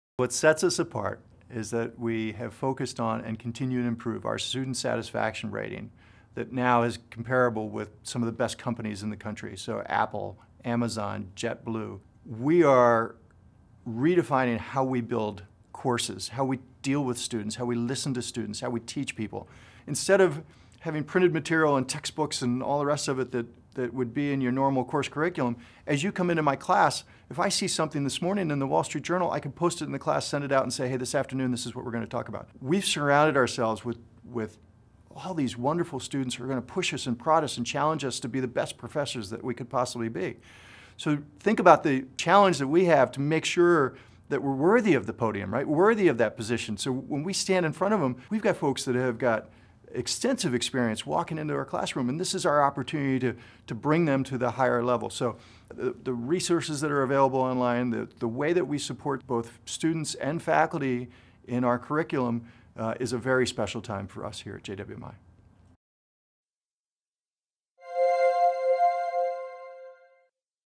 What sets JWMI apart. A message from Mike Zeliff, Dean of Faculty
"Our student satisfaction rate is comparable - or better - than some of the best companies in the world."